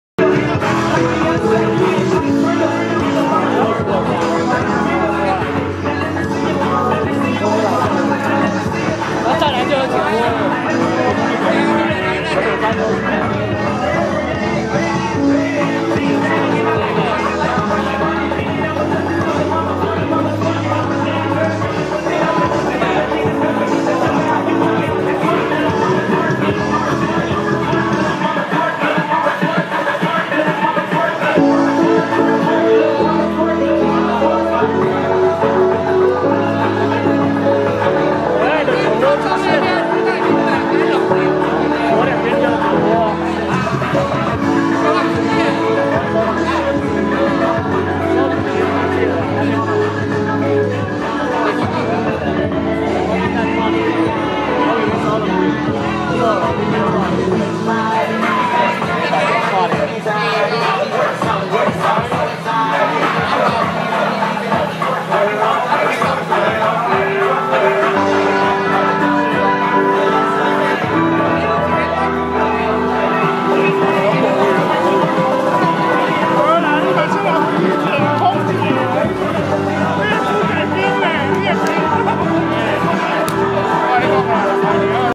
105丙申年北投慈后宮大拜拜繞境~【乩童拔口針退駕】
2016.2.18北投慈后宮恭迎天上聖母遶境{年例,北投大拜拜}